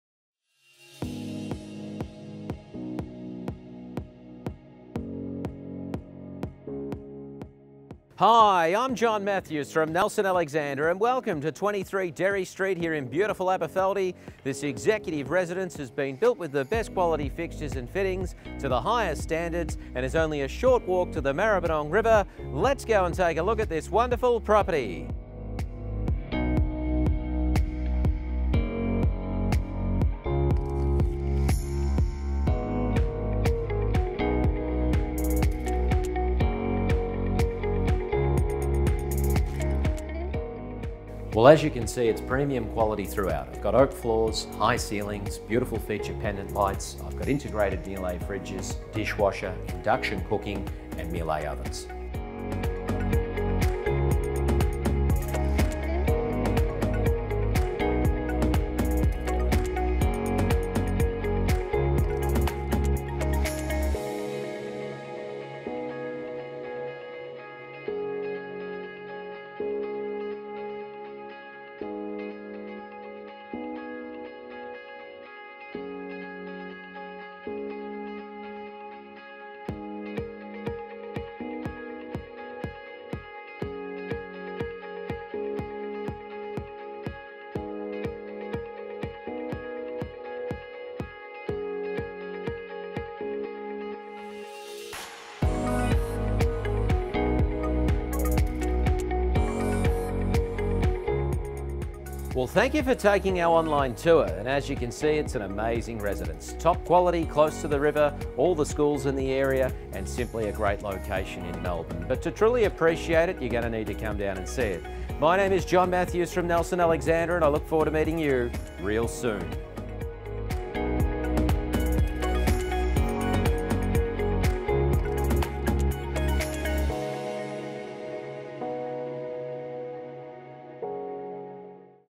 23 Derry Street, Alberfeldie For Sale by John Matthews of Nelson Alexander
John Matthews on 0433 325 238

Enviably positioned in an exclusive Aberfeldie streetscape, this remarkable residence delivers an as-new lifestyle of luxury and versatility with its spacious two-storey design, meticulous attention to detail and first-class features throughout. A creative combination of bold architecture and timeless elegance, the home's refined interior boasts four large bedrooms (alternate masters on each level with WIRs and deluxe ensuites), together with an equally-opulent main bathroom and separate powder rooms on each level. Highlighting its floorplan flexibility is a study area/retreat (upstairs) and expansive open-plan living/dining area accompanied by a superb kitchen complete with stone benchtops and built-in dining table, custom cabinetry, butler's pantry and premium Miele appliances (induction cooktop, oven, microwave, dishwasher and integrated fridge/freezer). Along with high square-set ceilings (3m downstairs/2.7m upstairs) further enhancing its sense of space, the home also includes a full-size laundry and excellent storage options, zoned ducted heating and refrigerated cooling, engineered oak flooring, double-glazed windows and soft-close cabinetry throughout. Complementing the home's sensational interior is a sunny courtyard (bi-folds from the living area) with entertaining deck and high fences for added privacy, plus an internally-accessed garage (remote-control) and additional driveway parking. Walking distance to the open spaces of Aberfeldie Park and endless trails along the Maribyrnong River, it's also close to the area's renowned primary and secondary schools, public transport options, local cafes and shops, as well as Clifton Park and Afton Street Conservation Reserve.